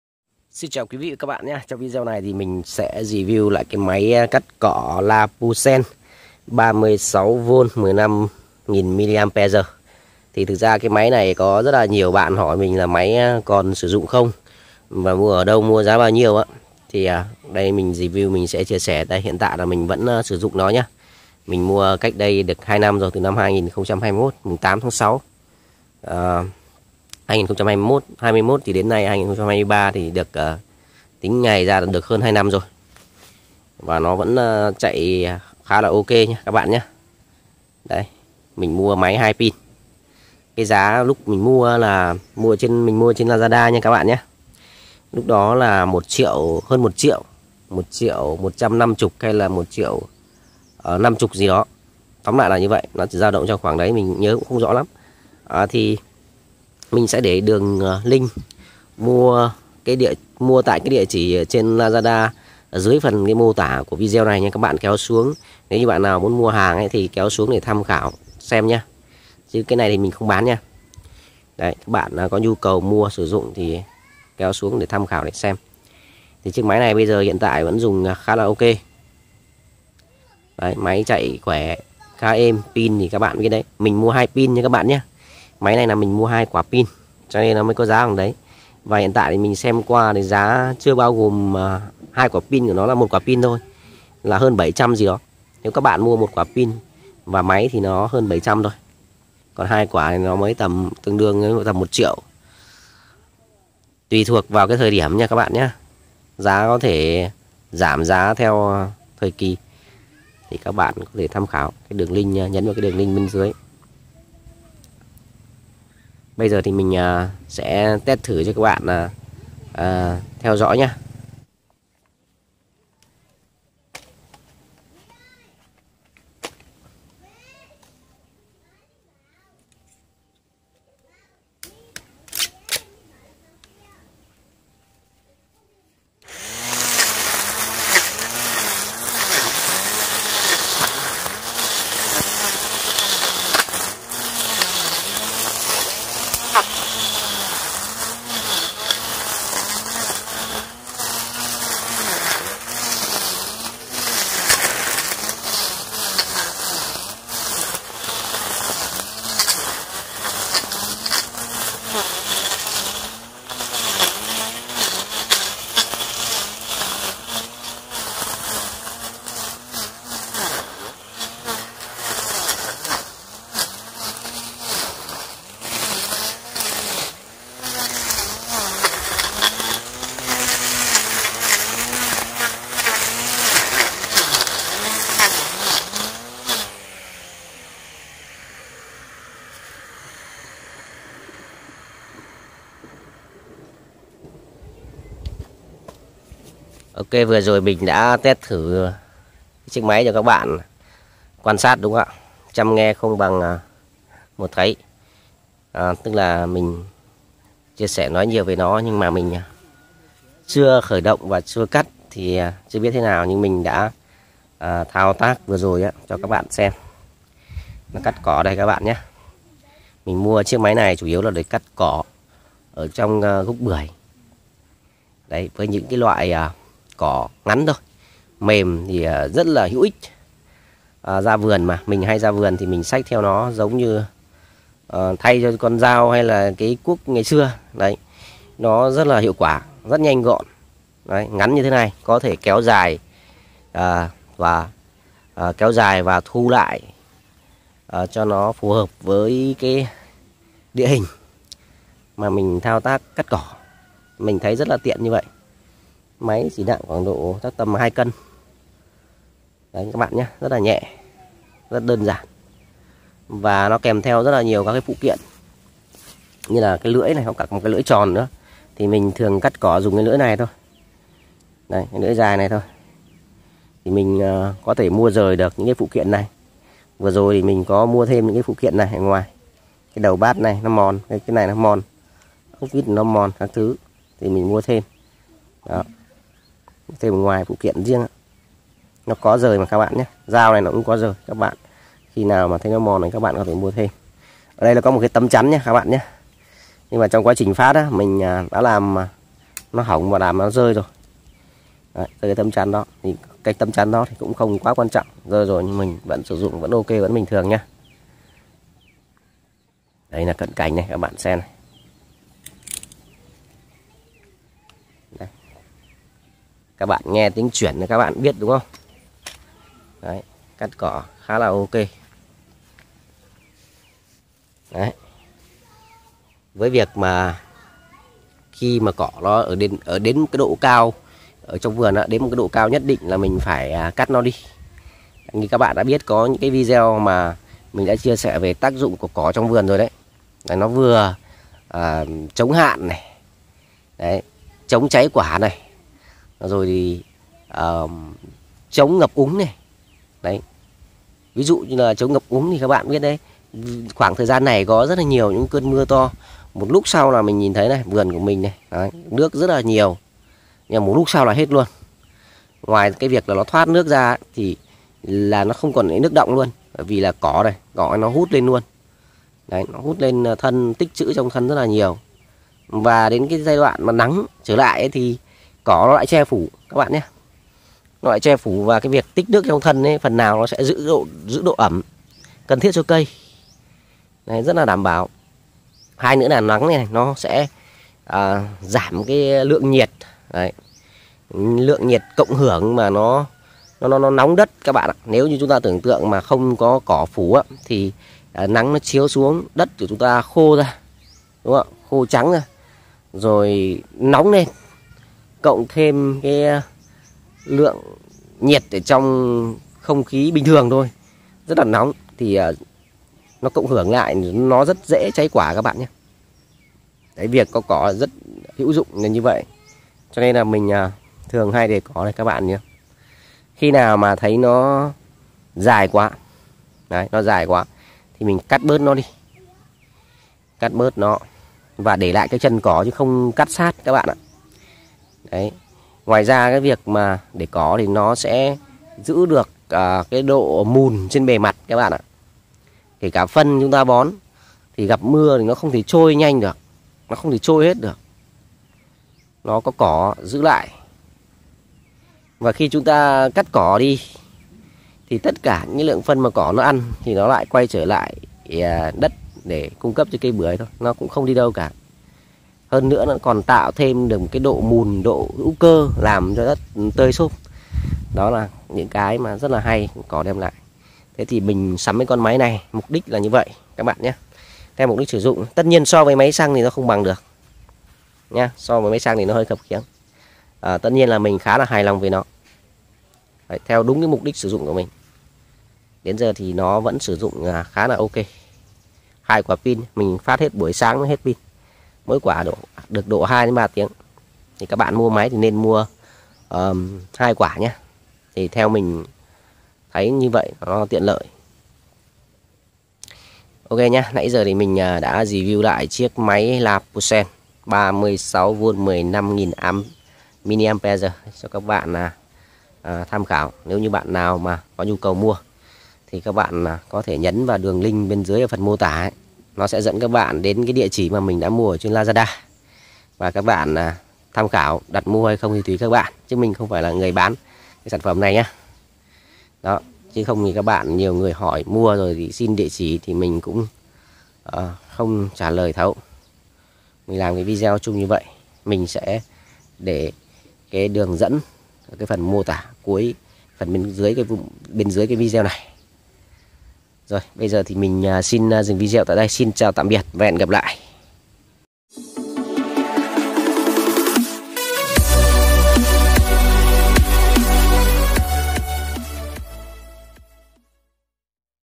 Lapusen máy cắt cỏ mini, mua ở đâu? bưởi Rỏn
✓ Trong Video này bưởi Rỏn chia sẻ tình hình chiếc máy cắt cỏ Lapusen. Chiếc máy này mình đã dùng hơn 2 năm, hiện tại nó vẫn chạy tốt. Các bạn thấy nó cần thiết so với nhu cầu của mình có thể nhấn vào link phía trên để tới lazada mua máy nhé.